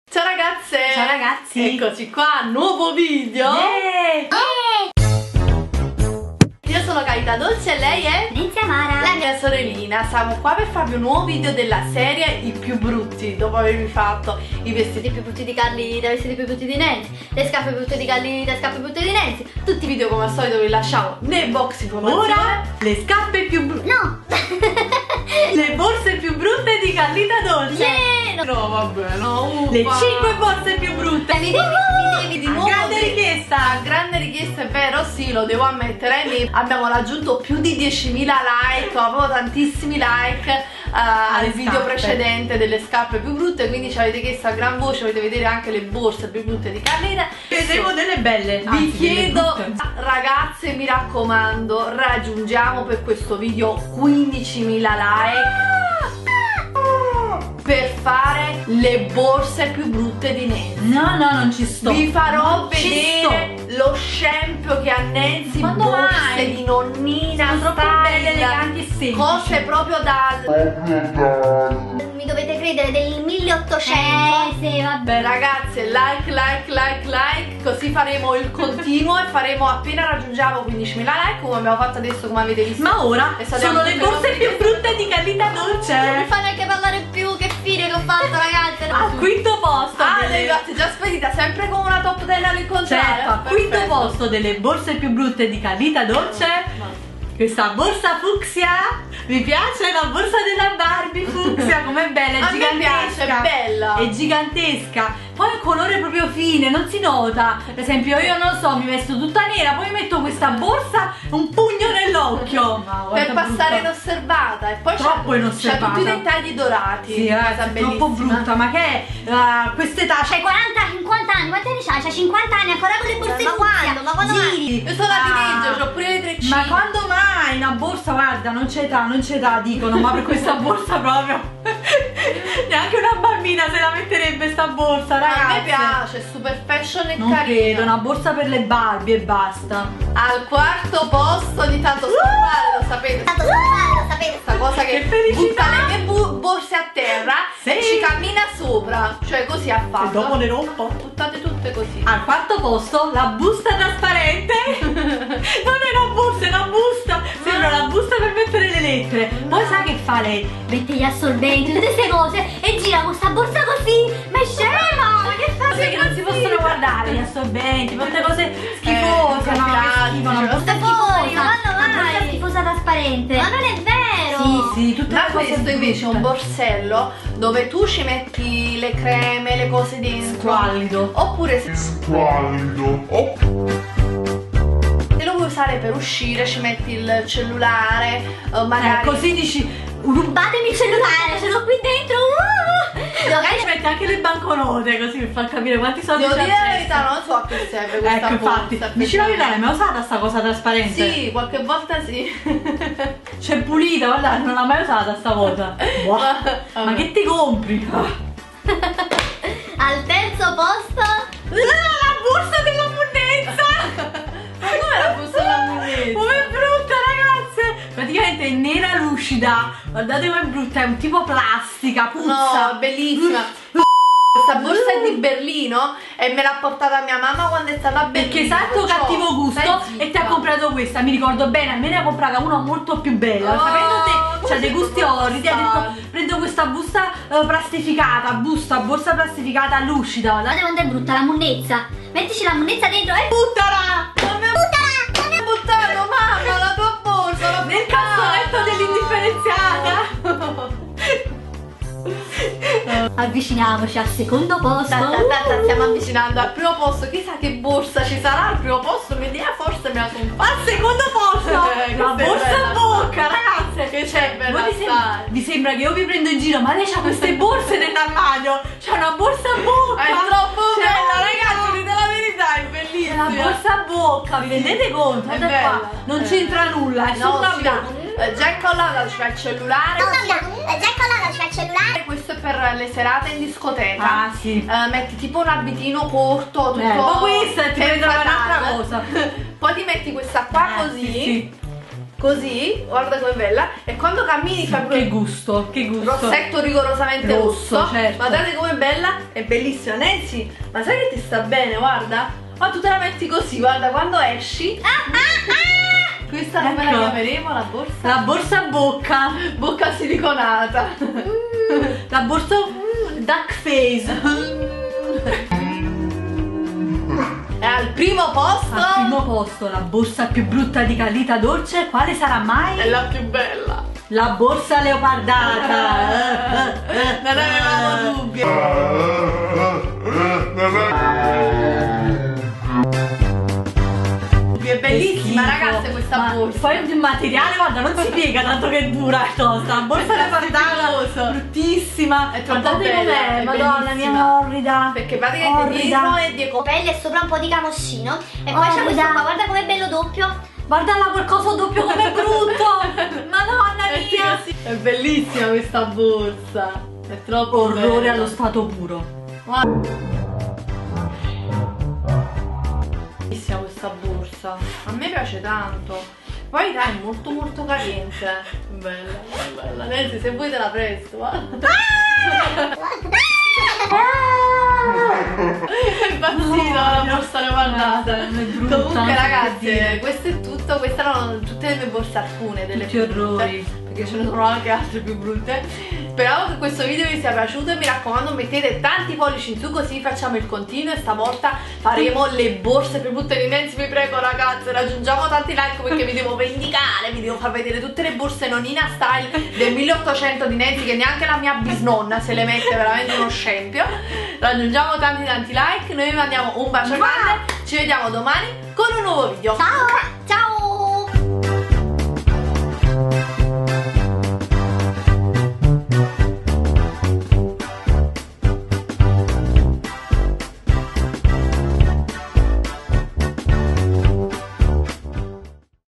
LE 5 BORSE PIÚ BRUTTE di CARLITADOLCE!!!..DISAGIO e IMBARAZZO!!!
Ciao ragazzeee!!! ^_^
Siamo tornate!! Dopo il Video sulle scarpe più brutte di Nancy ecco a vostra GRANDISSIMA RICHIESTA le BORSE più brutte di Carlita.. ragazze anche le borse di Nancy sono orrende!!! :-D Mi raccomando METTETE UN BEL POLLICE IN SU AL VIDEO se volete vederle... mi devo vendicare ;-) Raggiungiamo 15 mila LIKE per il Prossimo video della serie "i più brutti" ;-)

Grazie per i Vostri commenti, li leggiamo sempre tutti! *__*

Tante belle cose! ;-)

Carlita & Nancy ♡